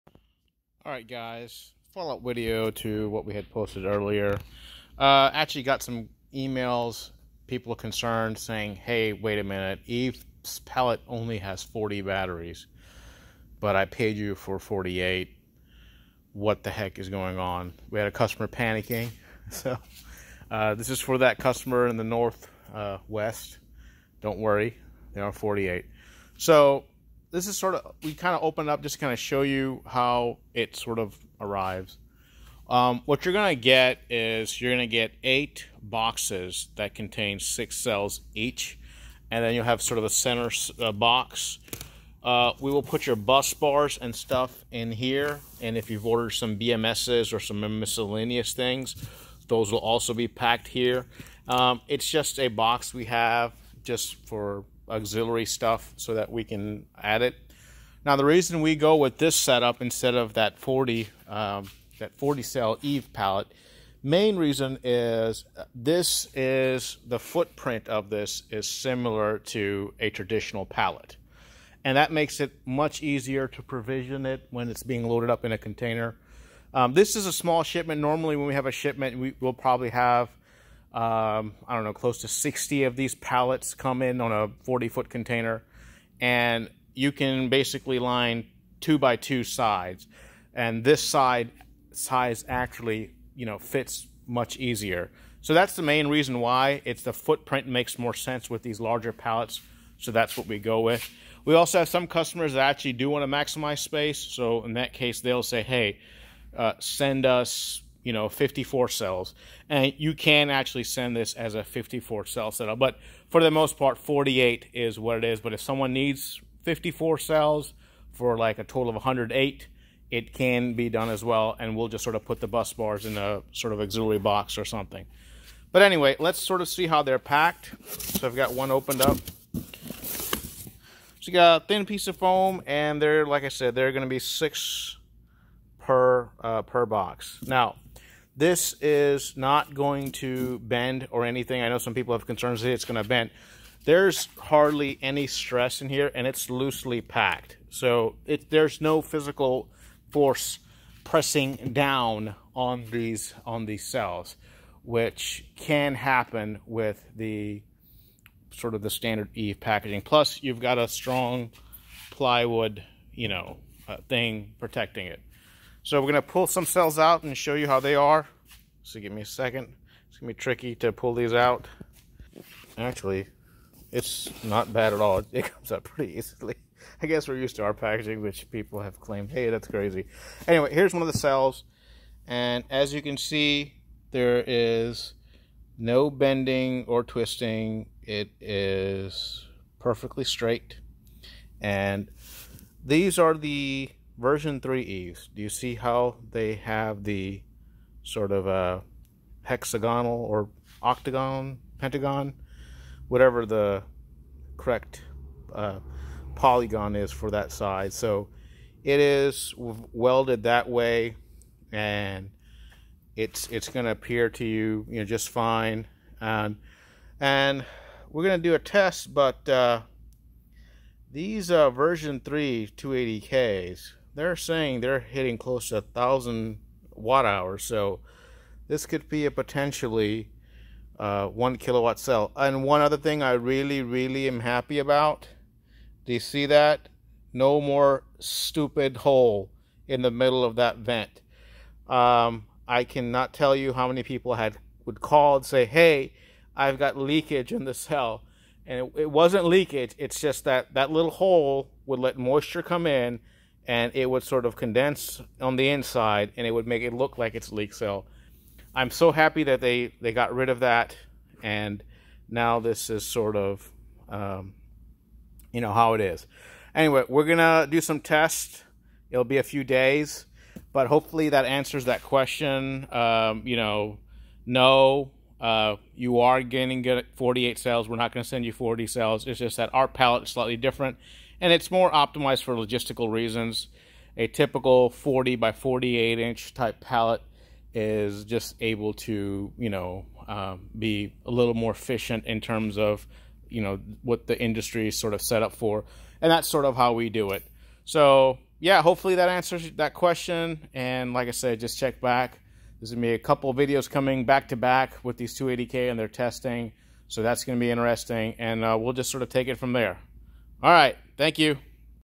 EVE 280K V3 Preview & Pallet Layout
Here is a preview of the EVE 280K V3 Cells & Pallet, they should be available on our site by Monday next week. Price for local warehouse is the same as the international warehouse.